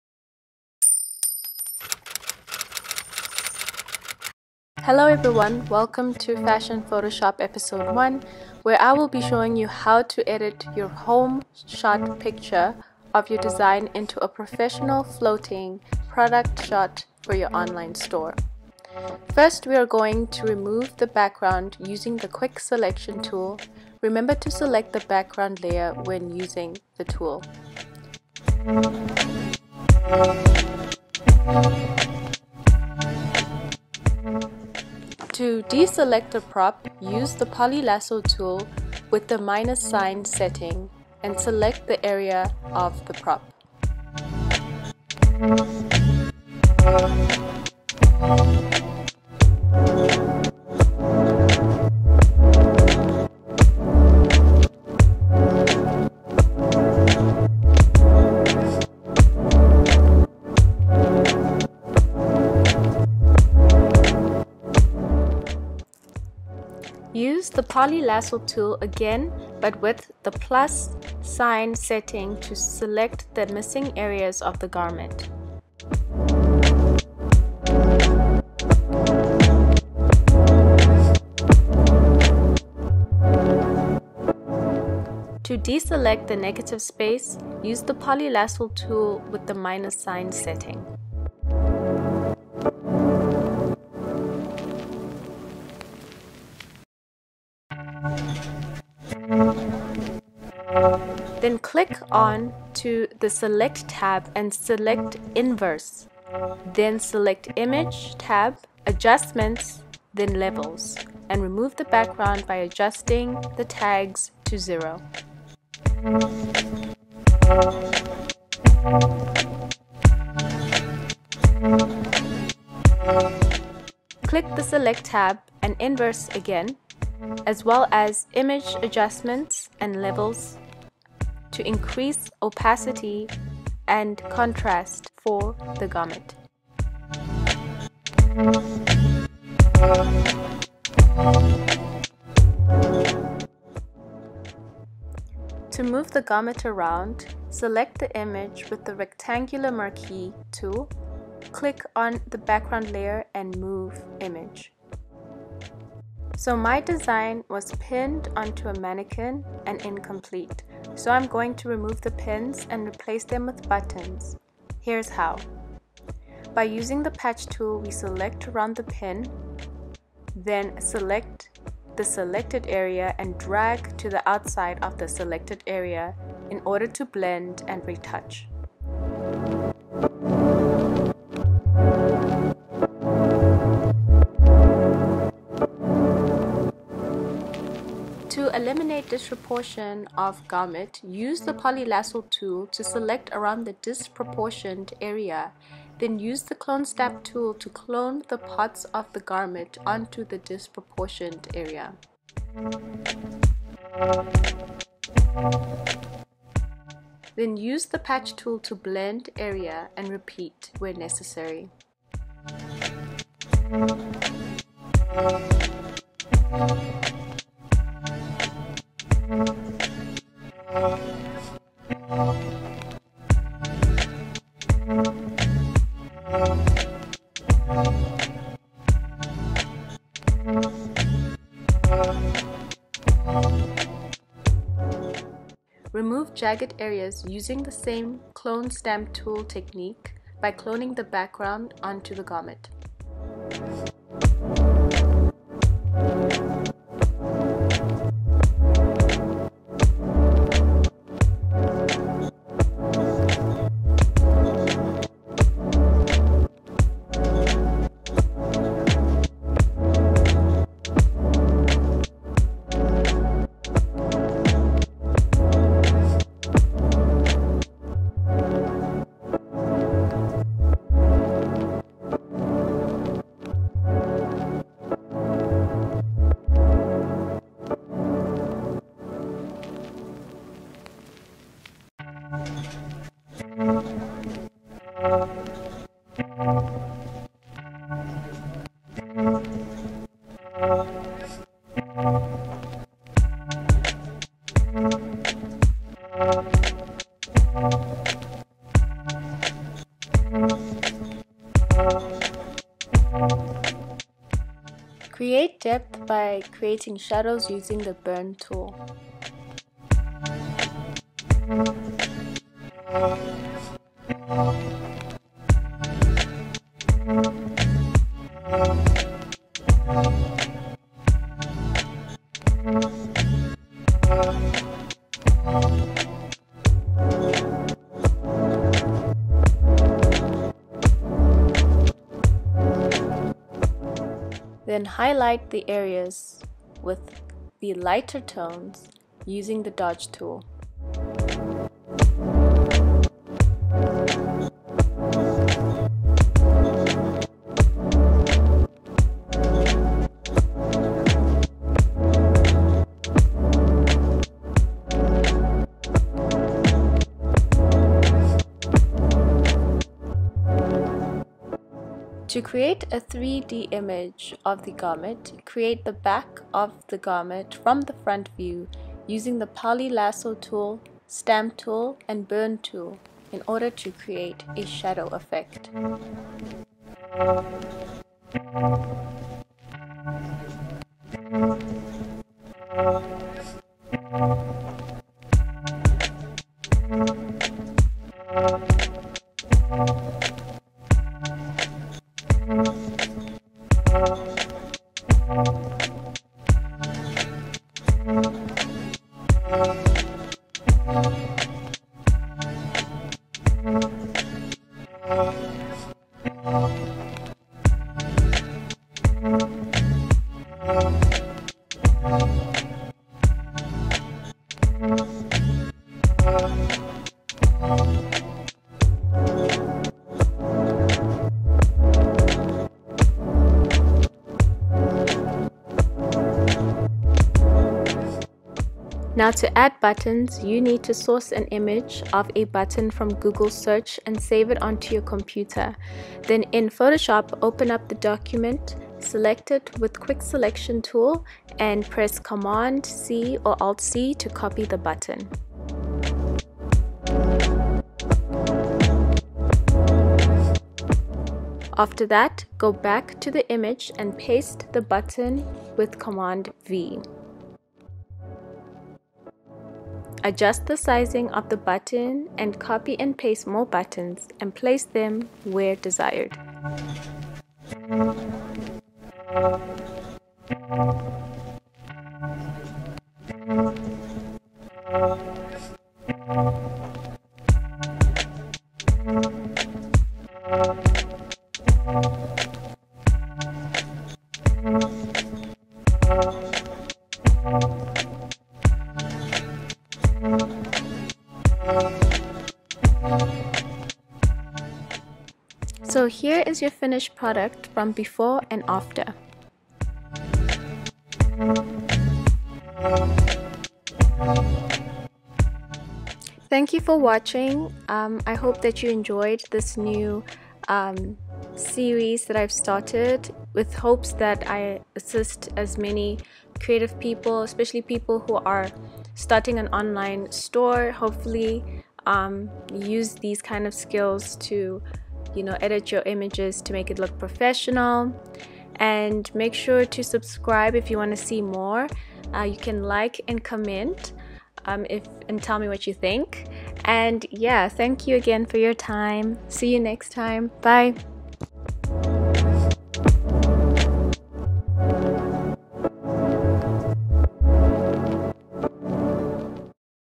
Fashion Photoshop | Creating Floating Product Image in Adobe Photoshop | EP.2
Hello Fashion Peeps!
In this Fashion Photoshop Series Tutorial, I will be showing you how to convert a home shot product image (shot in your living-room) into a professional 'floating' product shot, that could be used on your online e-commerce store.